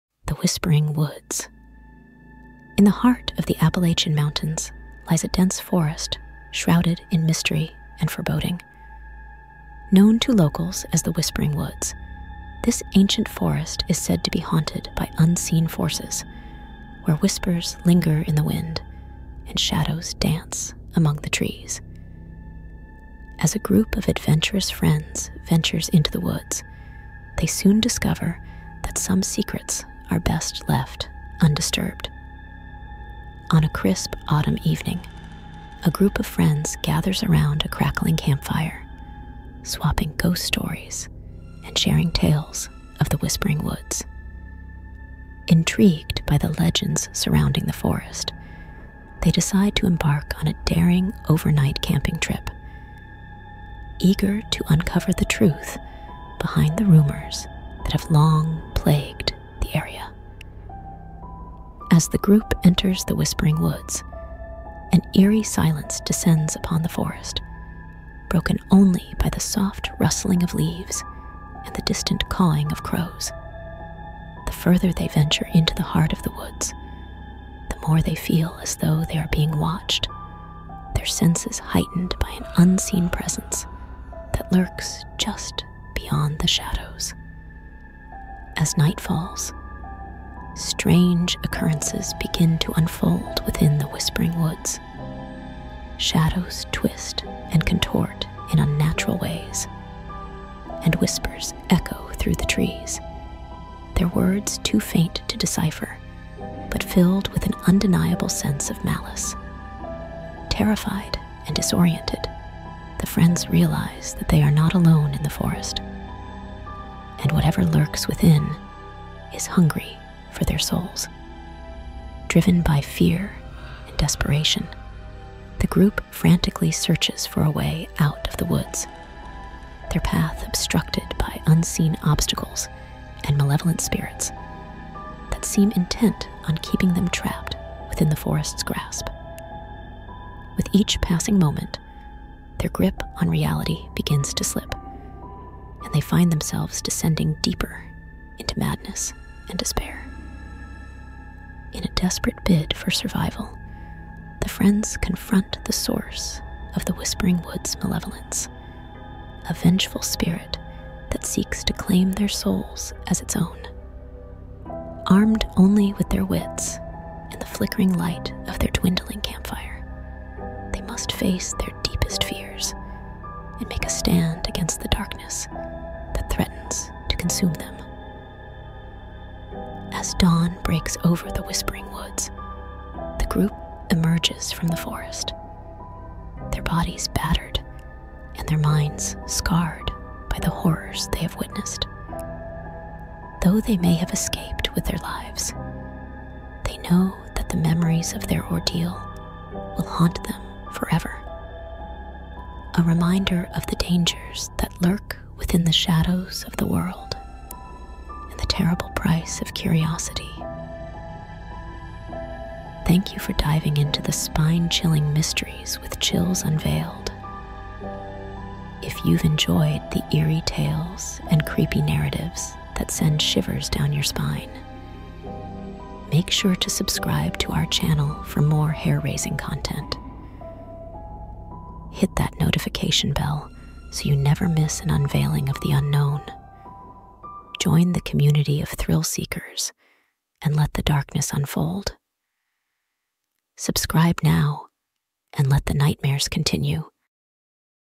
The Whispering Woods | An Original Horror Story
👻 **Synopsis:**
Embark on a spine-tingling journey into the heart of the Appalachian Mountains with "The Whispering Woods," where a group of friends ventures into an ancient forest haunted by unseen forces. As whispers linger in the wind and shadows dance among the trees, they soon discover that some secrets are best left undisturbed.

🌳 **Story Highlights:**

1. **The Invitation:** Join a group of adventurous friends as they gather around a campfire and decide to explore the mysteries of the Whispering Woods, eager to uncover the truth behind the legends that have long haunted the area.

2. **Into the Unknown:** Venture into the dense forest where an eerie silence descends, broken only by strange rustlings and unsettling whispers that seem to emanate from the shadows themselves.

3. **The Haunting:** Experience the terror as night falls and the forest comes alive with malevolent spirits and unnatural occurrences, driving the friends to the brink of madness as they struggle to escape the woods' grasp.

4. **The Descent:** Follow the group as they navigate through obstacles and confront the source of the Whispering Woods' malevolence, facing their deepest fears in a desperate bid for survival.

5. **The Final Stand:** Witness the friends' last stand against the darkness as they confront a vengeful spirit determined to claim their souls, fighting for their lives amidst the flickering light of their dwindling campfire.

If you're a fan of supernatural horror stories that delve into the mysteries of the natural world and the depths of human fear, "The Whispering Woods" offers a chilling tale that will keep you on the edge of your seat until the very end.

Dare to enter "The Whispering Woods" and uncover the secrets that lie within.